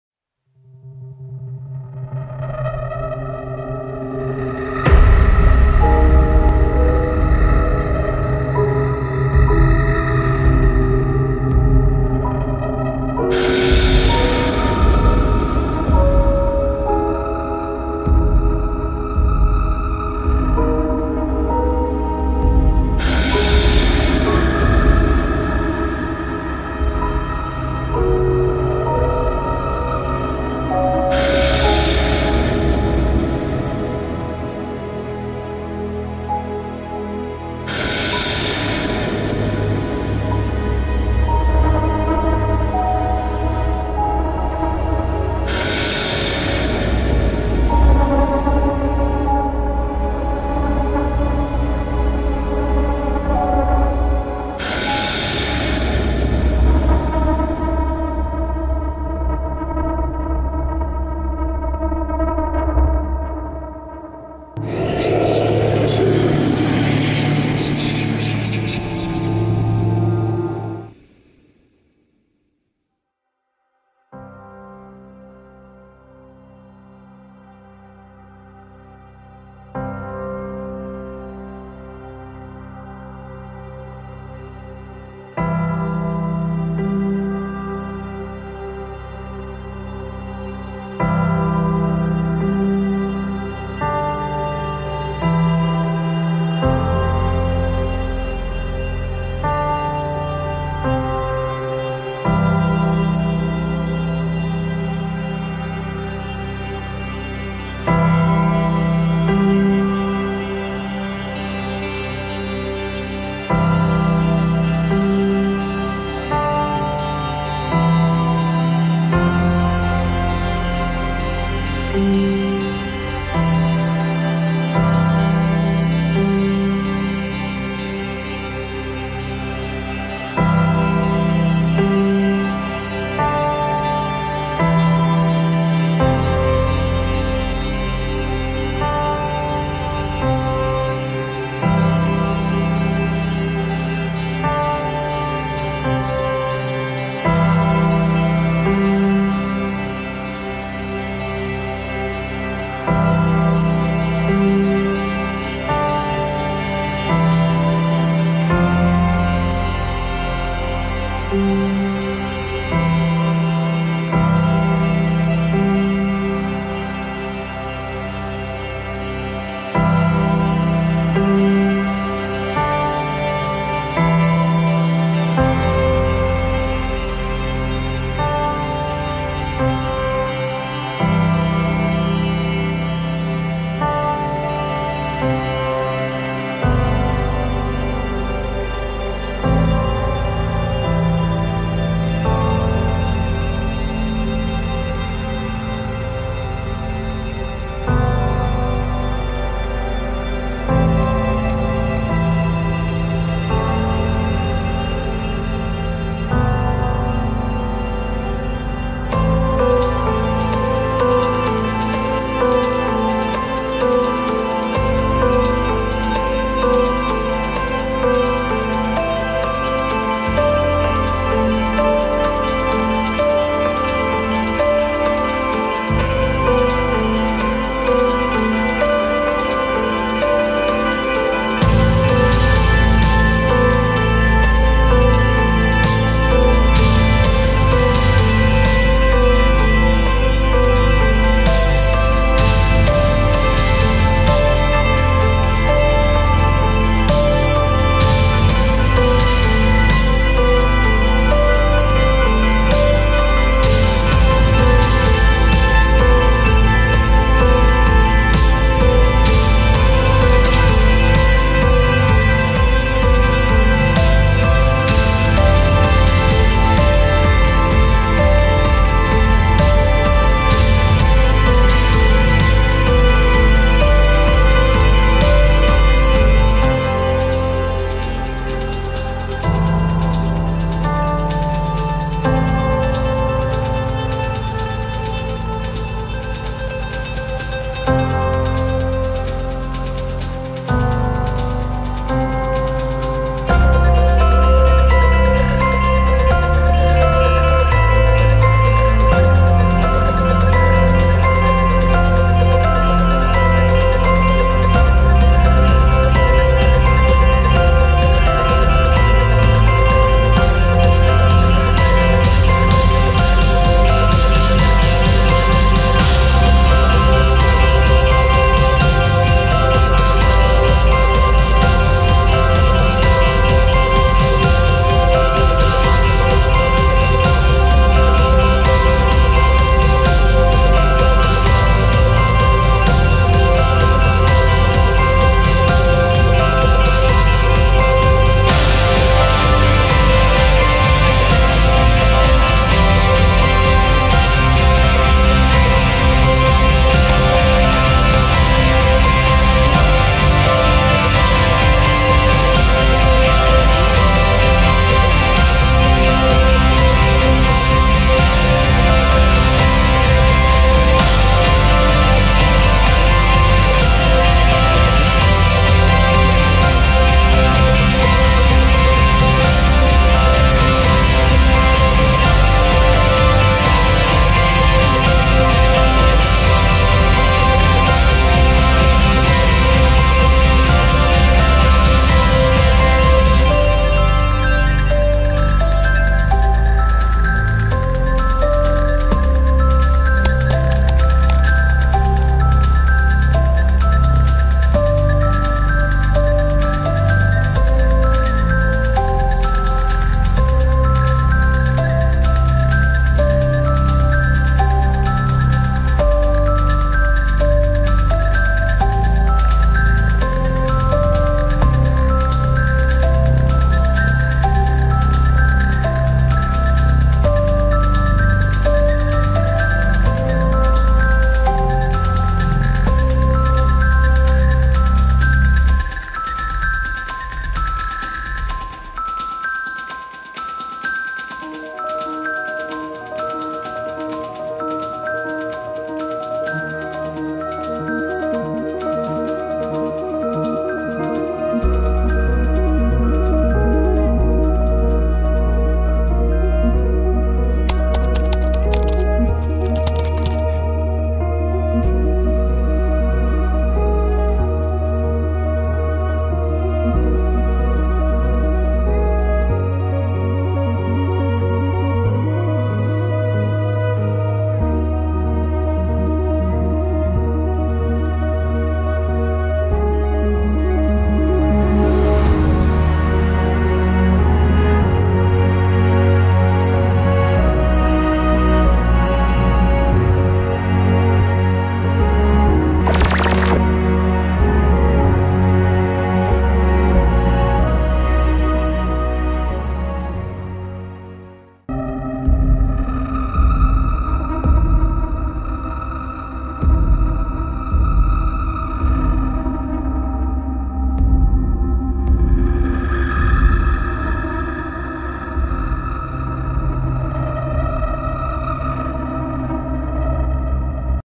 Afflicted: Merla's POV Prologue ~ part 2
Merla gets an urgent message from her best friend.